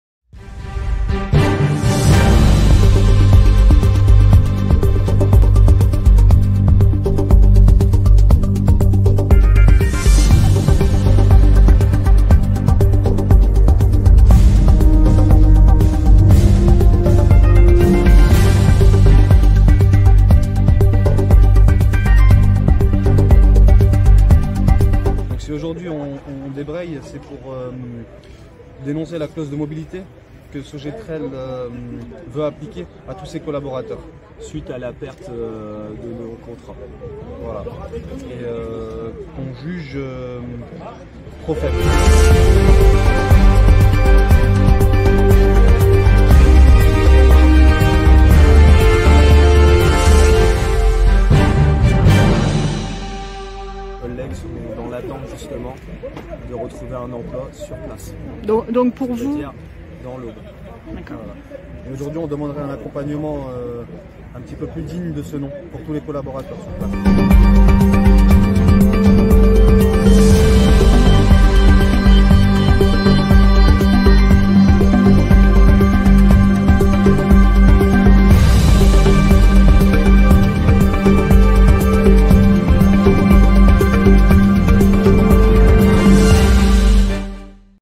Buchères : les salariés de Sogetrel se refusent à déménager pour garder leur emploi
Une cinquantaine de salariés de Sogetrel ont débrayé ce lundi matin devant leurs locaux à Buchères. Ils protestent contre une clause mobilité nationale jugée abusive.